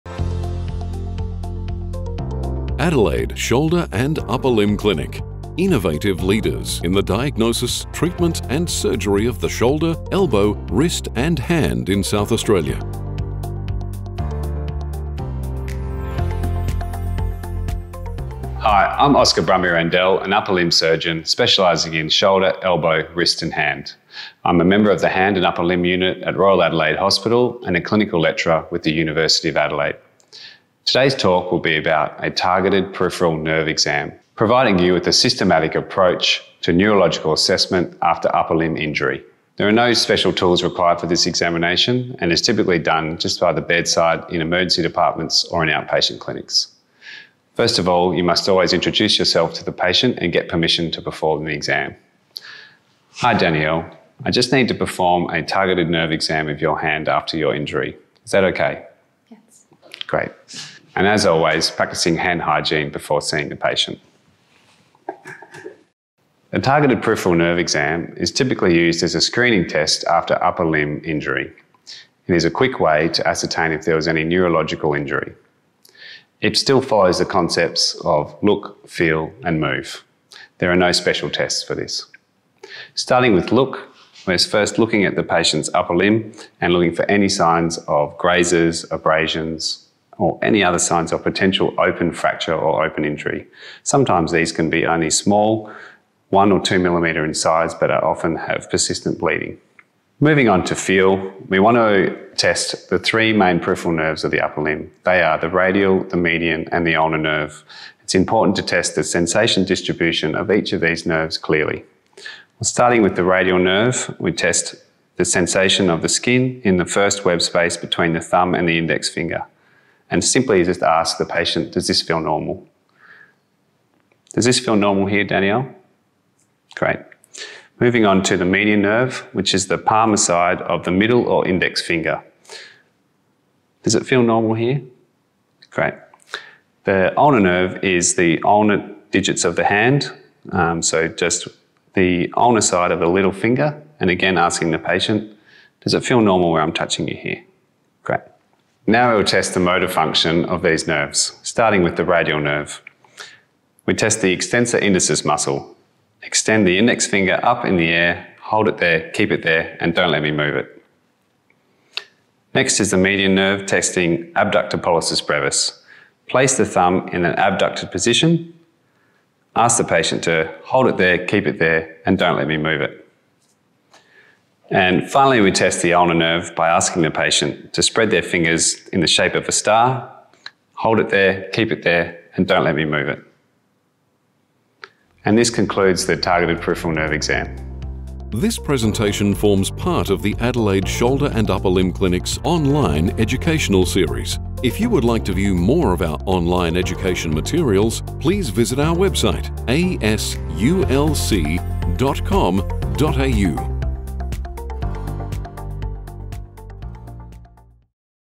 How To: Peripheral Nerve Examination. By an Orthopaedic Surgeon.
In this video, Dr Oscar Brumby-Rendell takes us through a peripheral nerve examination.

The aim of this video is to demonstrate a systematic approach to neurological assessment after upper limb injury.

There are no special tools required for this examination, and it is typically done by the bedside in emergency departments or in outpatient clinics.

Timestamps:
01:06 What is the test used for?
01:22 Look
01:40 Feel
02:28 Move